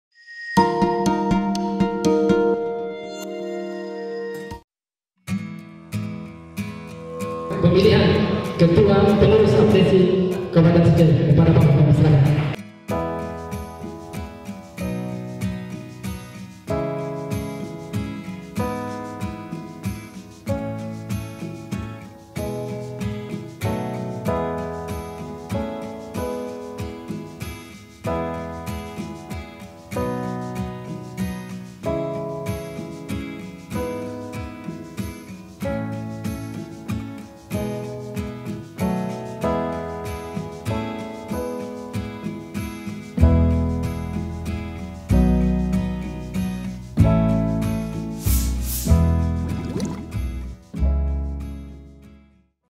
Andi Asis Soi Pimpin APDESI Sinjai Periode 2018-2022
SULSELSATU.com, SINJAI - Musyawarah Cabang (Muscab) Asosiasi Pemerintah Desa Seluruh Indonesia (APDESI) Kabupaten Sinjai yang pertama kali digelar akhirnya memilih ketua untuk periode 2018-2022.

Pada muscab tersebut, terpilih Kepala Desa Bua, Kecamatan Tellulimpoe, A. Asis Soi sebagai Ketua APDESI Sinjai. Ia terpilih secara aklamasi dalam Muscab yang digelar di Gedung Hotel Hawai Sinjai, Senin (13/8/2018) itu.

Sejatinya, ada tiga kepala desa yang mencalonkan diri dalam Muscab APDESI pertama ini. Namun setelah berembuk, kedua calon lainnya memberikan amanah kepada A. Asis Soi untuk memimpin APDESI hingga empat tahun mendatang.

Ketua DPW APDESI Sulsel, Sri Rahayu Usmi bersyukur muscab APDESI Sinjai bisa terlaksana dan memilih ketua untuk periode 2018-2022.

"Teman-teman APDESI Sinjai sangat luar biasa yang memegang rasa silaturahim. Saya mengucapkan terimakasih dan jalinan silaturahim ini terus dijaga. APDESI Sinjai mantap dan sangat luar biasa," katanya.

Penulis: Andi Irfan Arjuna
Video Editor: Andi Hermanto
Editor: Awang Darmawan